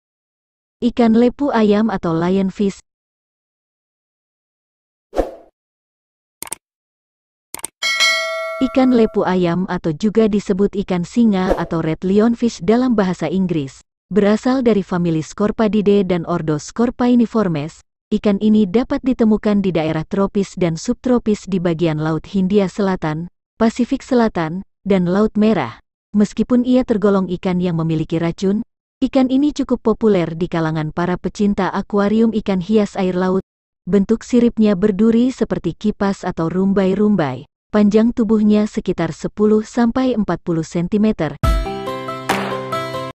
Ikan Lepu Ayam (Lion Fish) Ikan Mematikan Yang Sangat Cantik
Ikan Lepu Ayam ( Lion Fish )

Lepu adalah sekelompok spesies ikan laut yang beracun yang tergabung dalam genus Pterois, Parapterois, Brachypterois, Ebosia atau Dendrochirus dari keluarga Scorpaenidae. Lepu merupakan spesies predator. Ketika berburu, mereka akan menyudutkan buruannya dengan sirip besarnya dan dengan refleks cepatnya mereka menekan buruannya sekaligus. Lepu dikenal karena durinya yang panjang dan memiliki warna merah, coklat, oranye, kuning, hitam atau putih berselang-seling. Kelompok ikan ini diklasifikasikan sebagai sebuah subfamili (Pteroinae) atau sebuah suku di bawah Scorpaeninae (Pteroini).

@bang korep animal